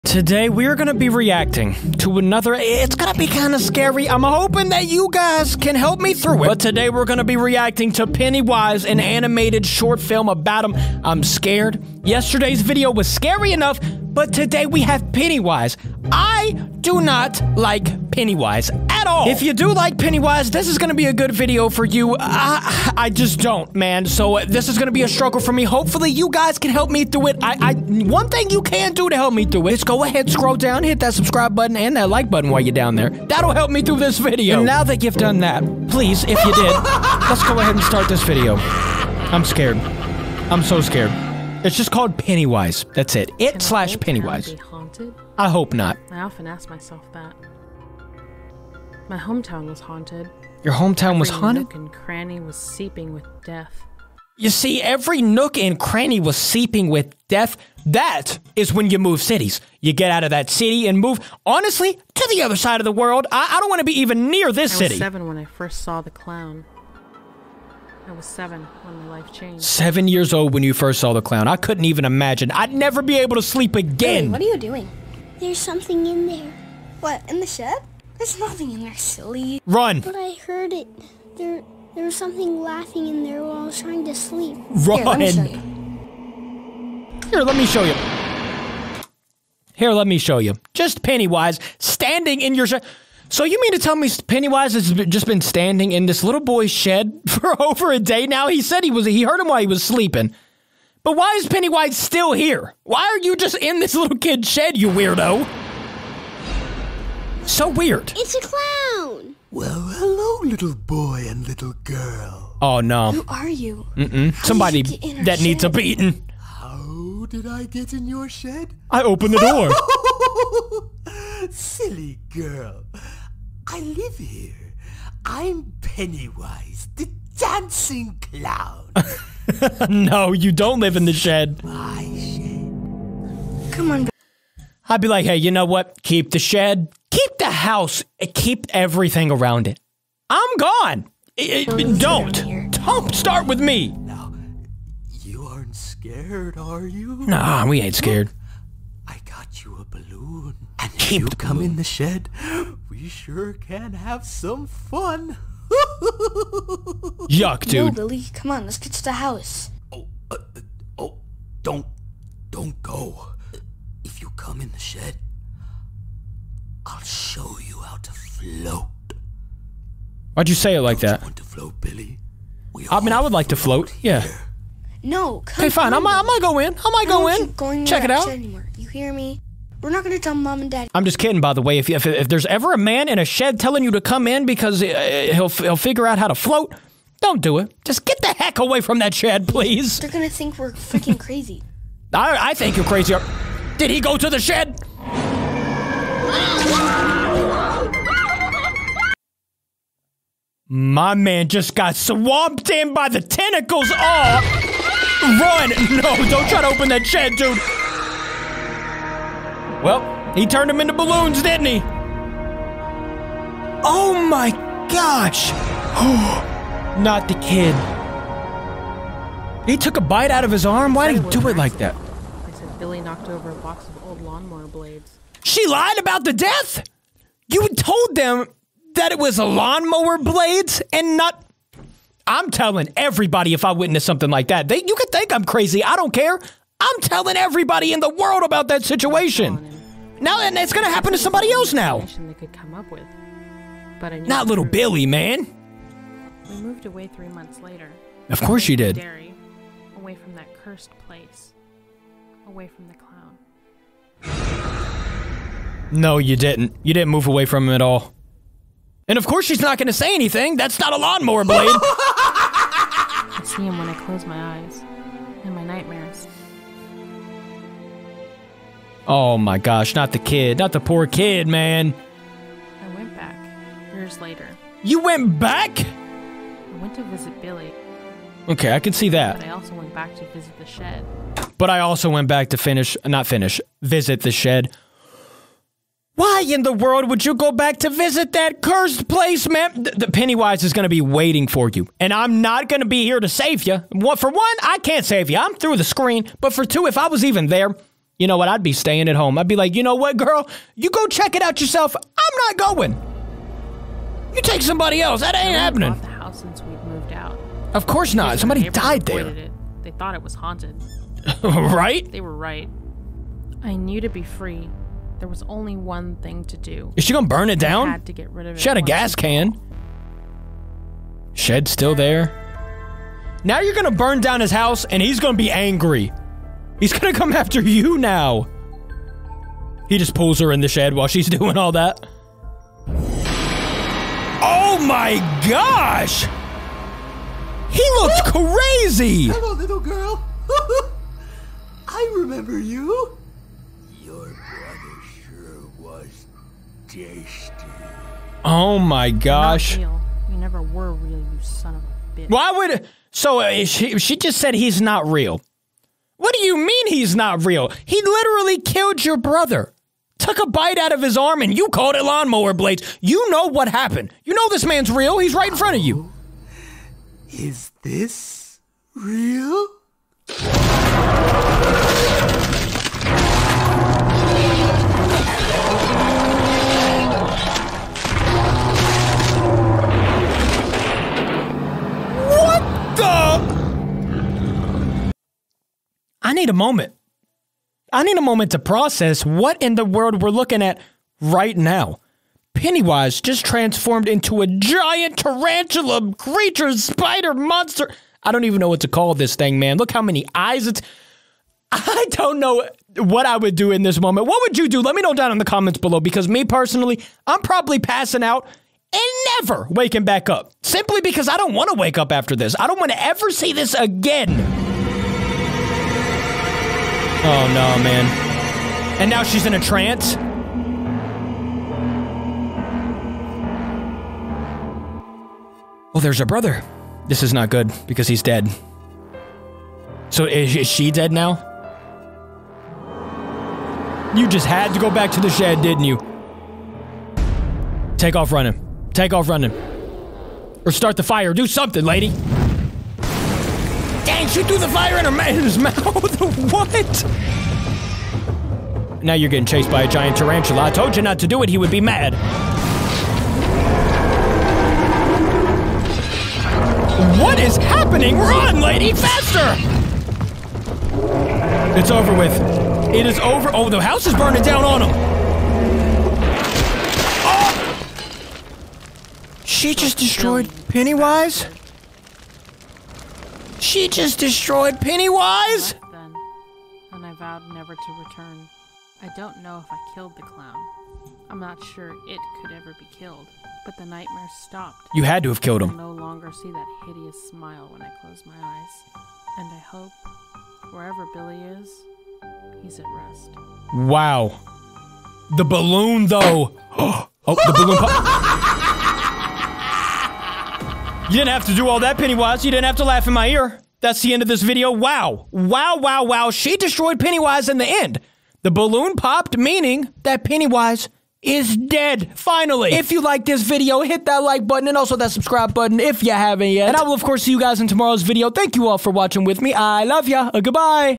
PENNYWISE WAS FOUND IN THIS SHED!!
They found Pennywise in their shed and they couldn't believe the outcome!! The dancing clown is not one to be messed with!!